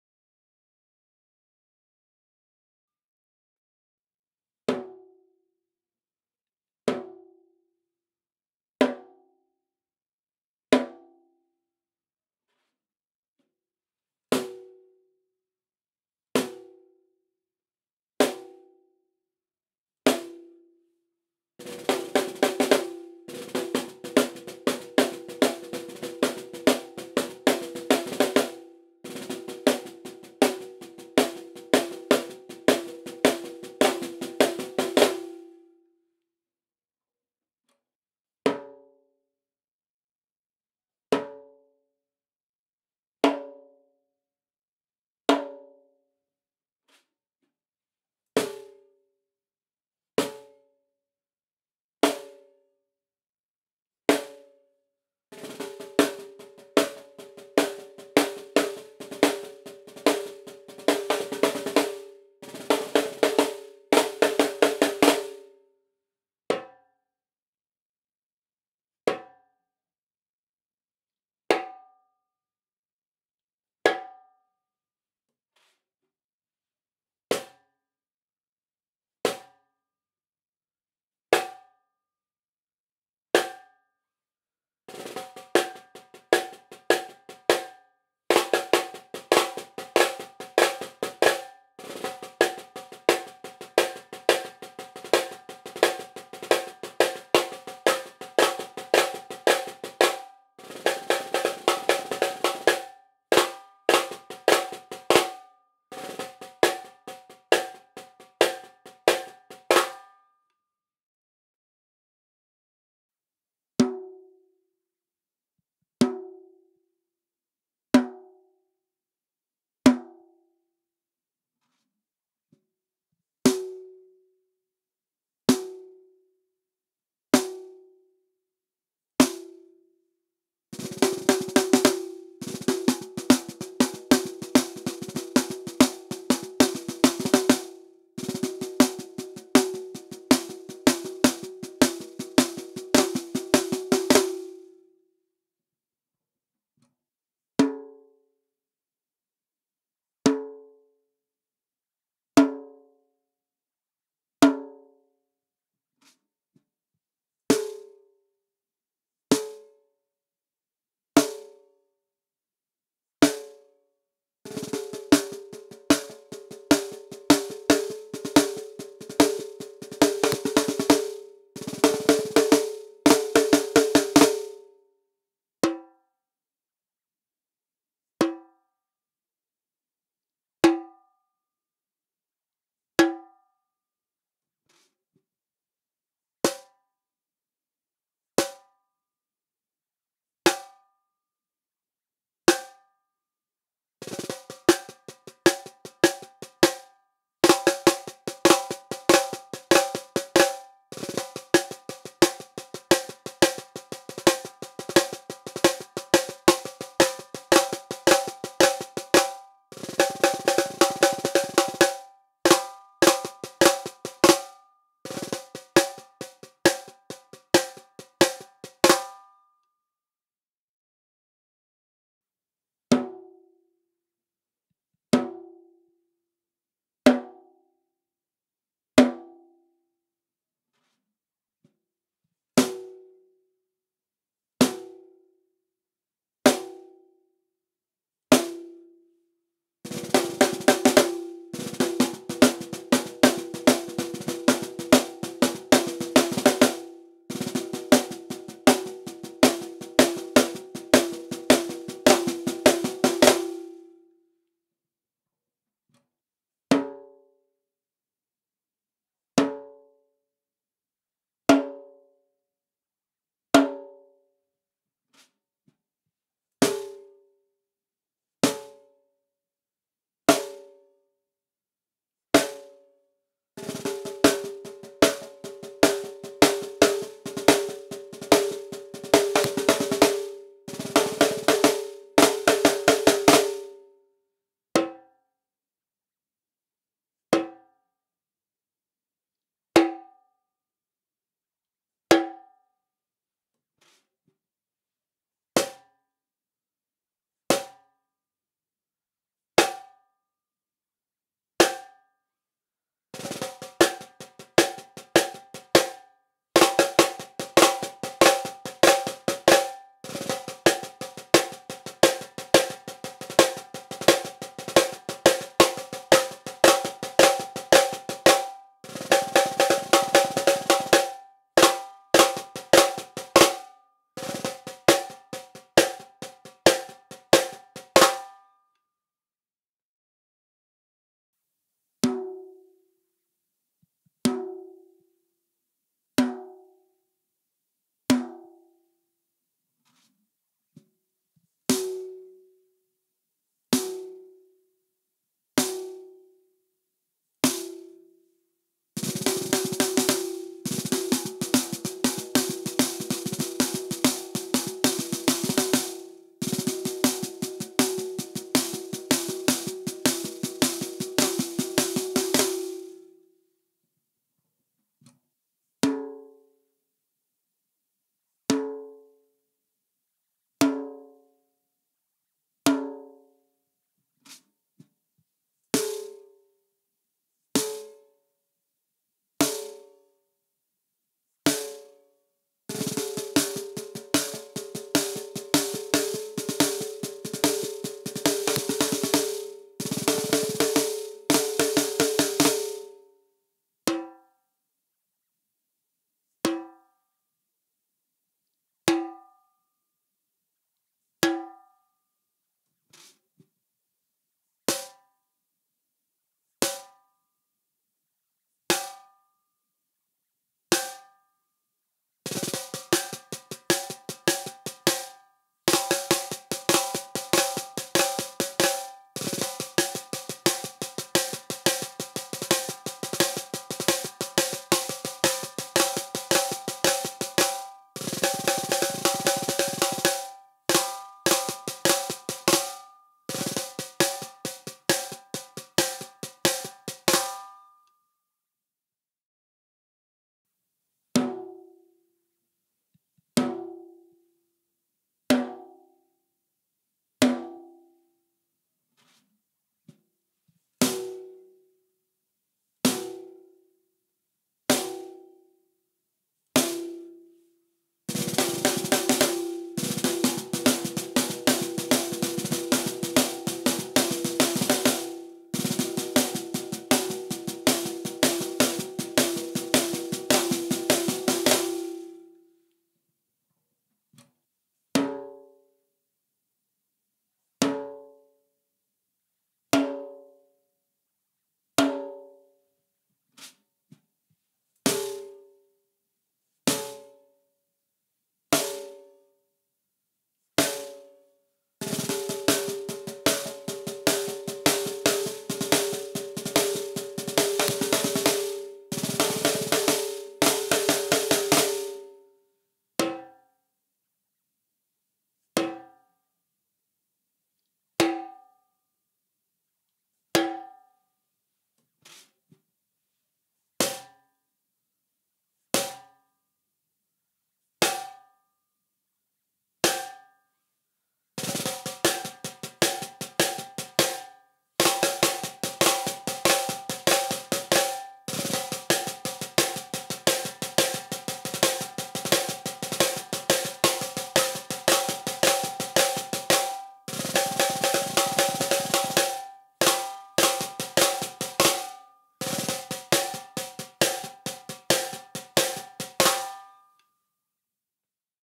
Kustom 6.5"x10" Stainless Steel in Three Different Tune Five audio Mode
Kustom 6.5"x10" Stainless Steel in Three Different Tune Five audio Mode. Recorded With Zoom H4n, Shure SM57 and Sennheiser MD 421